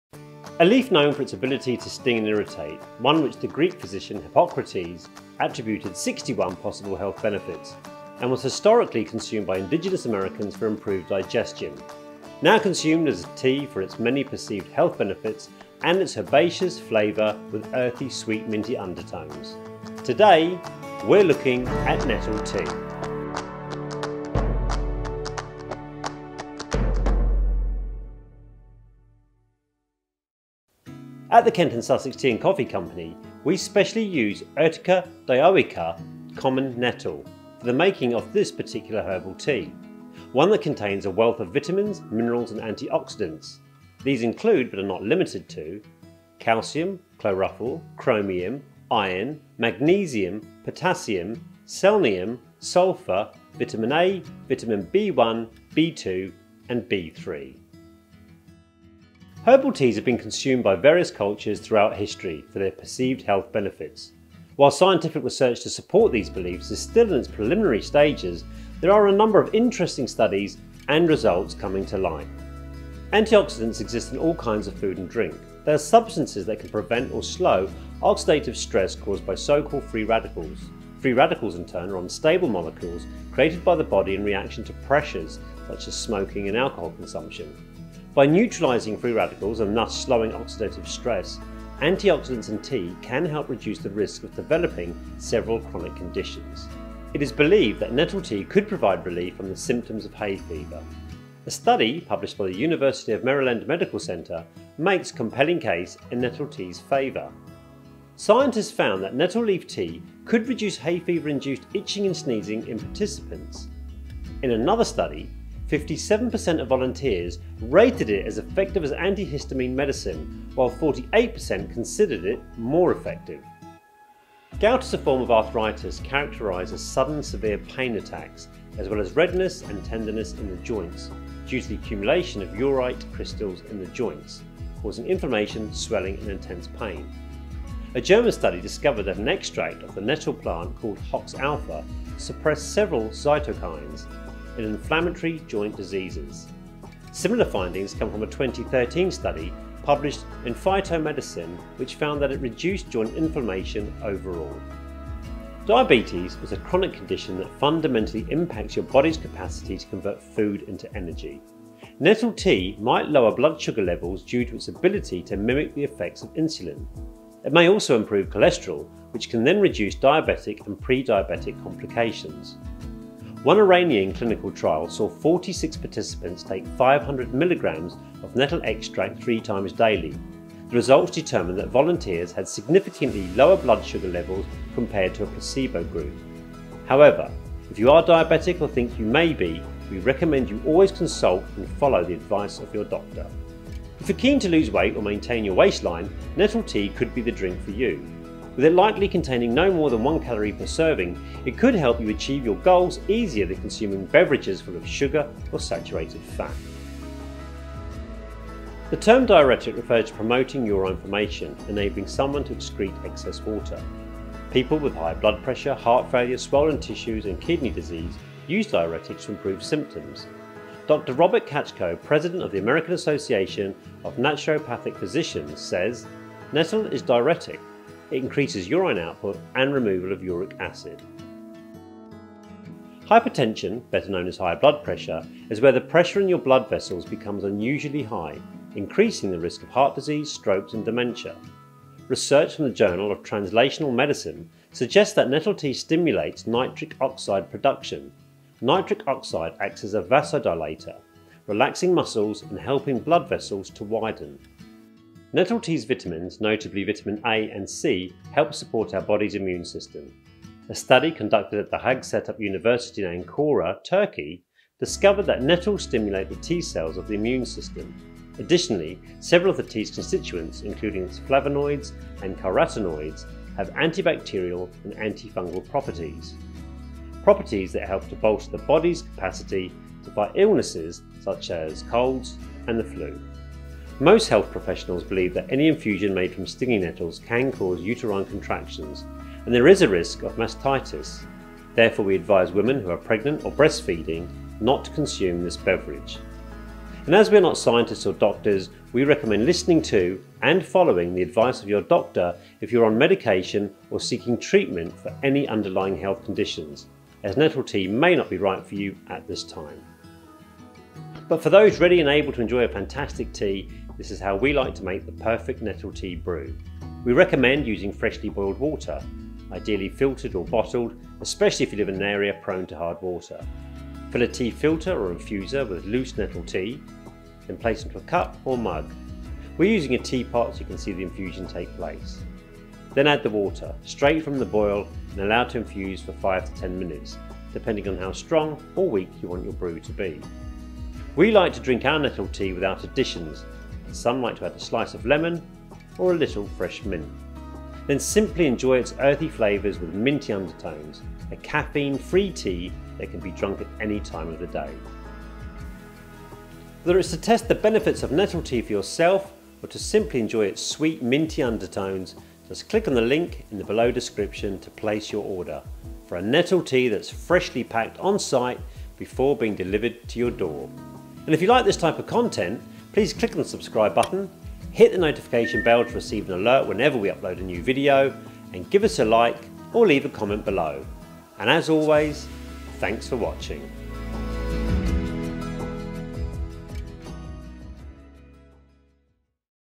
How to Make Nettle Tea, What are the Benefits
We love to hate Nettles. It’s something we’ve done for millennia. But should we loathe them when, according to evidence, they provide Nettle Tea benefits?

Perhaps it’s time for a change. Perhaps it's time to discover their full potential in the following video. Learn the facts, figures, history and health benefits of Nettle Tea including a guide on how to make Nettle Tea at home.